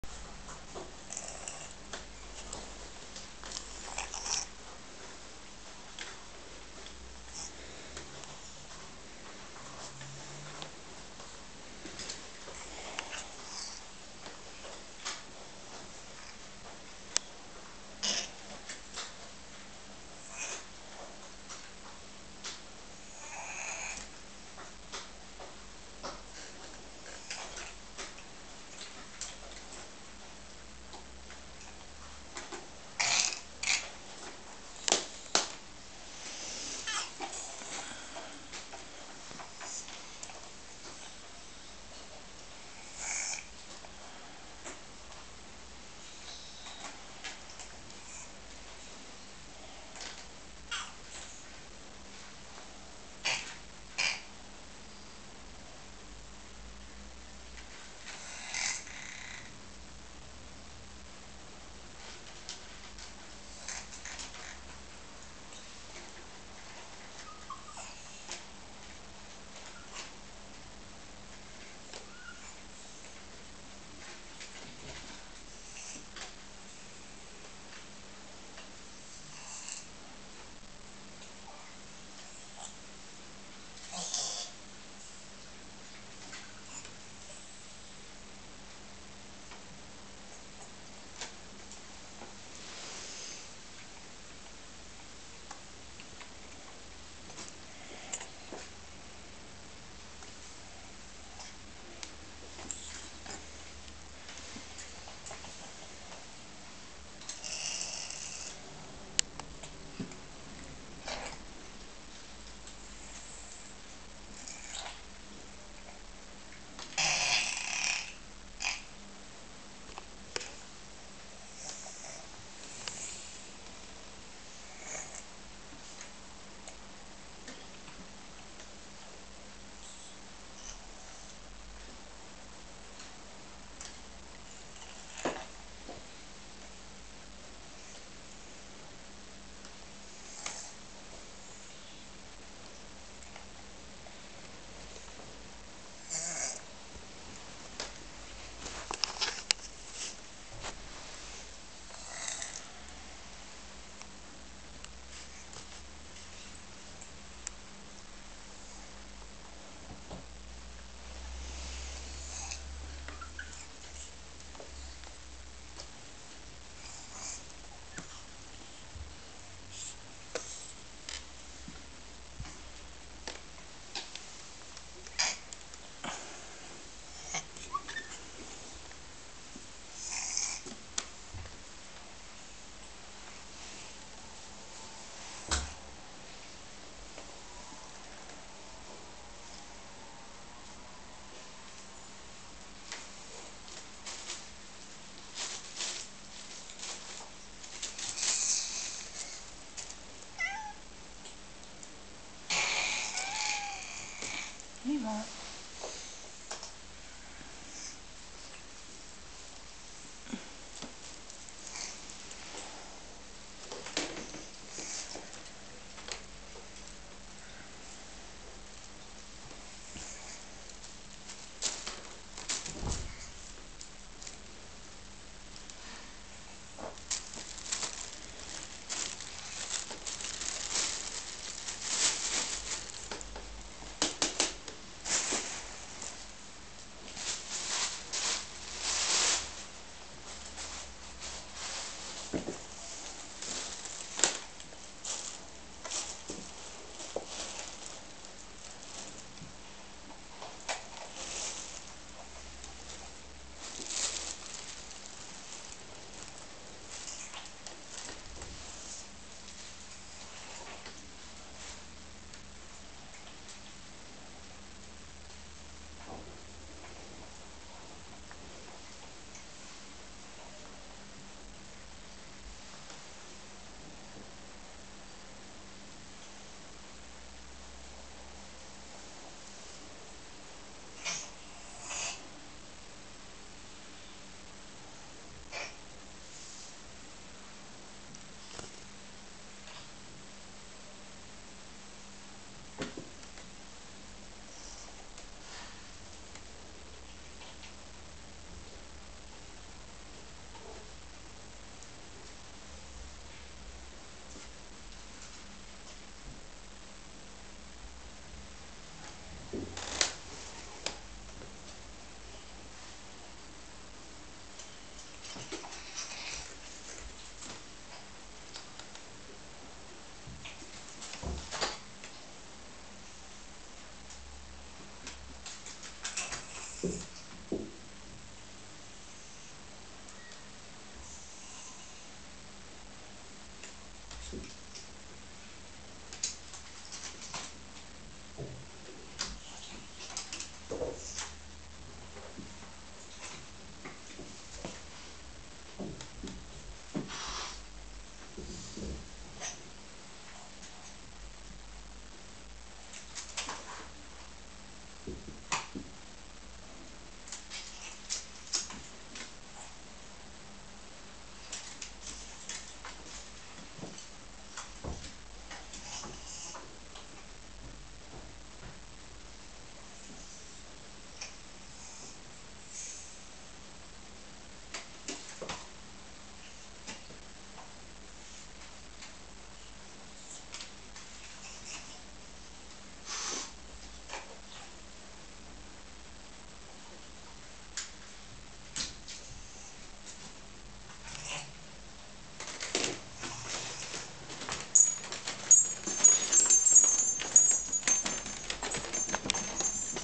2009-01-27 (01) En la entrada
He llegado a casa hace unos minutos. Han venido a recibirme y decido ver que hacen